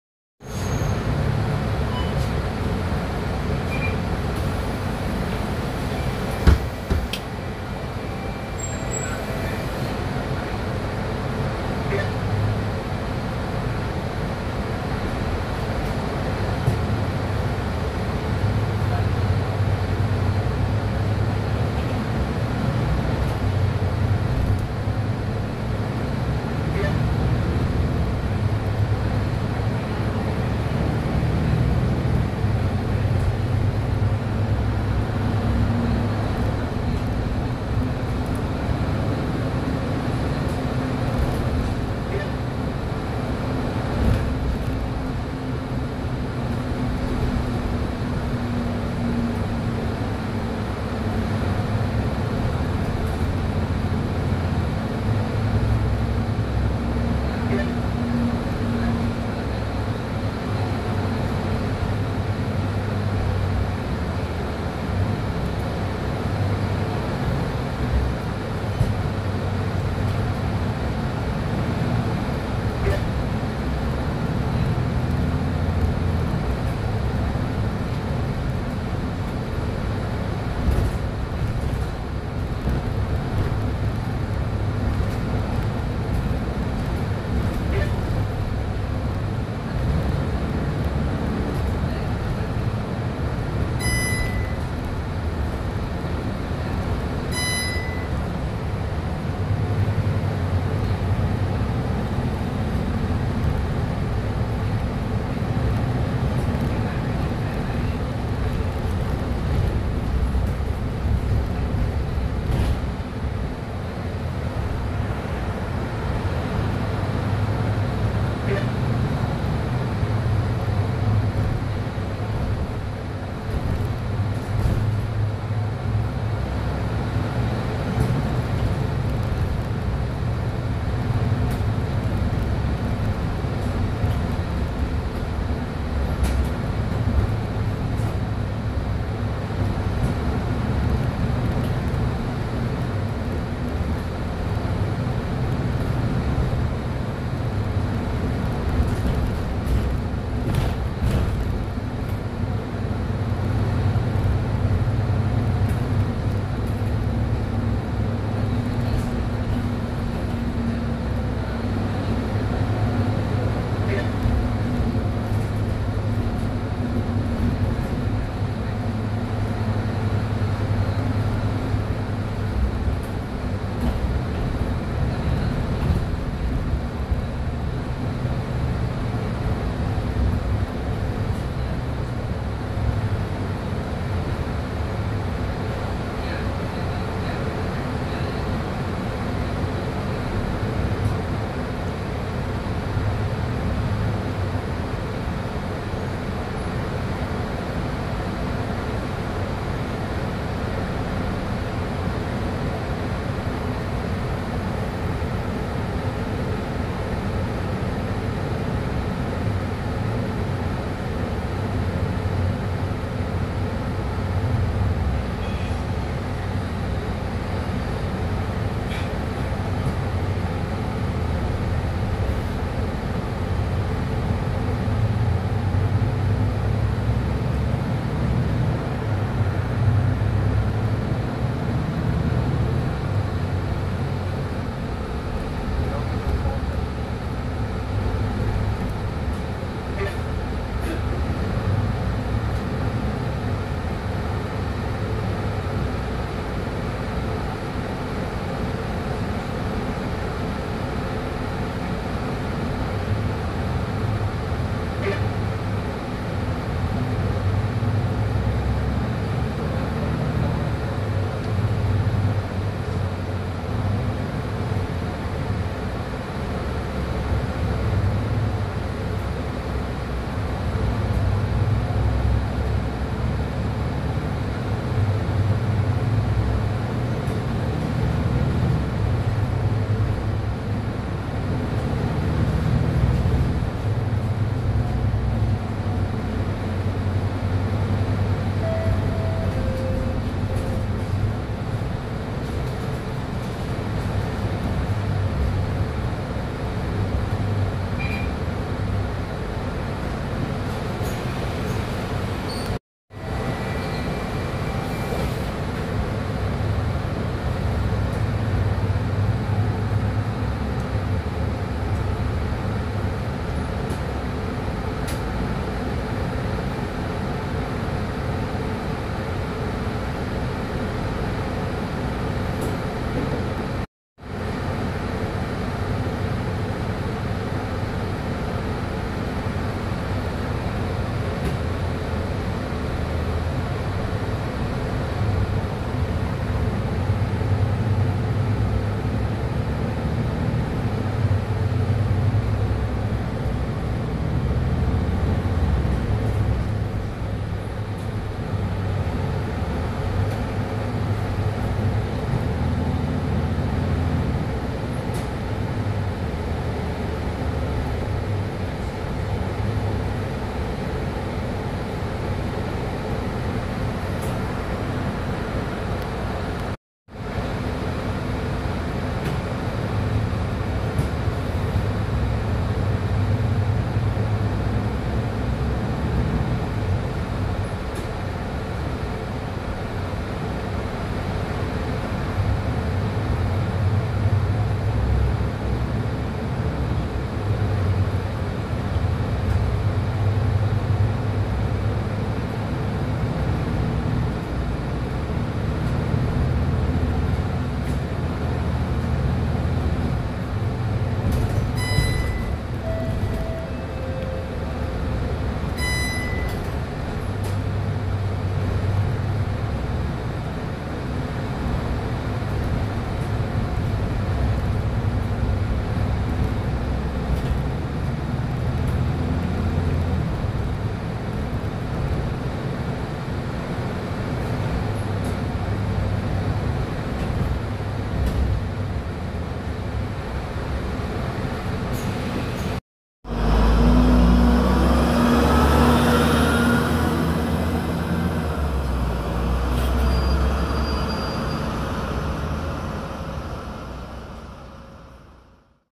SBST Bus Services 162 Mercedes-Benz O530 Citaro Batch 3
Registration: SBS 6592 U

Temporary Route Amendment

City Area Formula 1 2019 Singapore Grand Prix – Bus Diversions and Public Transport Plans